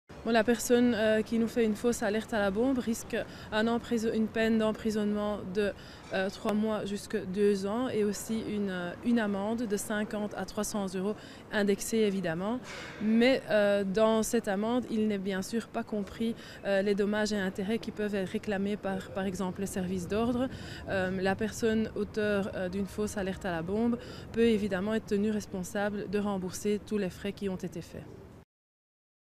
Enquête ouverte sur les différentes alertes à la bombe de jeudi
Le parquet de Bruxelles ouvre une enquêtes sur les différentes alertes à la bombe de ce jeudi à Bruxelles. Aucun suspect n’a été identifié et aucun lien n’a pu être établi entre les différentes alertes pour le moment. La journée de jeudi était, en effet, très chargée. Une première alerte a eu lieu en matinée sur le site de Mercedes à Woluwe-Saint-Lambert. Une seconde a conduit à l’évacuation des campus de la VUB d’Etterbeek et de Jette. Une dernière en fin d’après-midi concernait la salle de concert Forest National. (Interview : Bruzz)